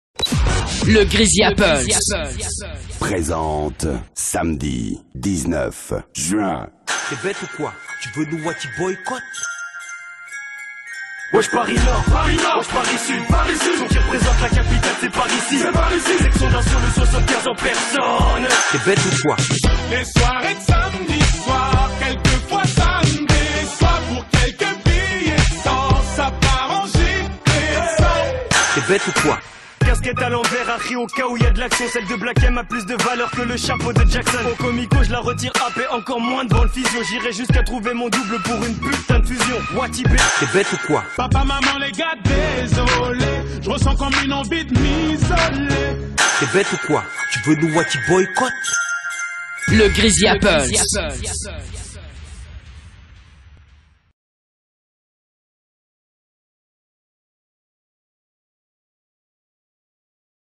SEXION D ' ASSAUT @ DISCOTHEQUE GRISY APPLE ' S
SAMEDI 19 JUIN

SEXION D ' ASSAUT ( la révélation rap français de l'année )
EN SHOWCASE LIVE

ENTREE 12 EUROS
INFO : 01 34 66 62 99
MICHOKO : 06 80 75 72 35